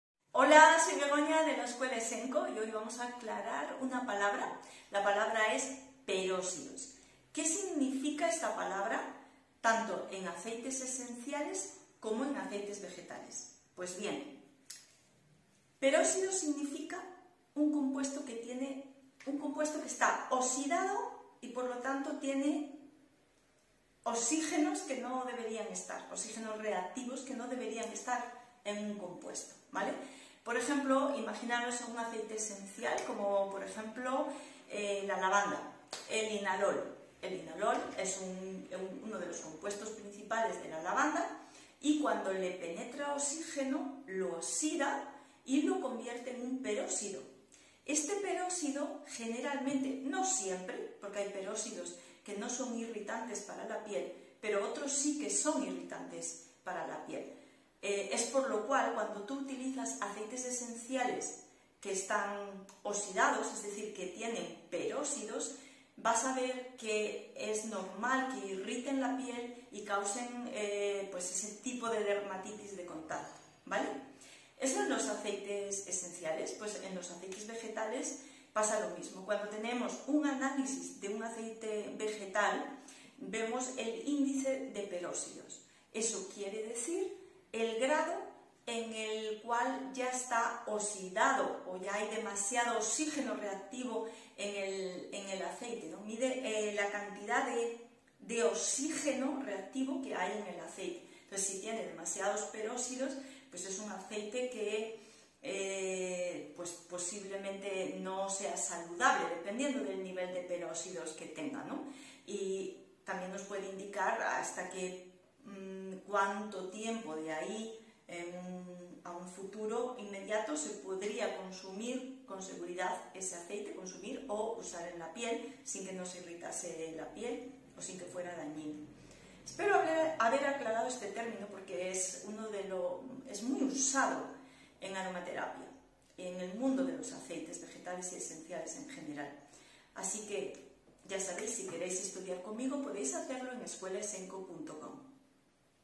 VIDEO#377 PERÓXIDOS EN LOS ACEITES.
Los peróxidos nos dicen que el aceite está oxidado. Puede ser muy oxidado o poco oxidado. Los peróxidos pueden causar irritación cutánea.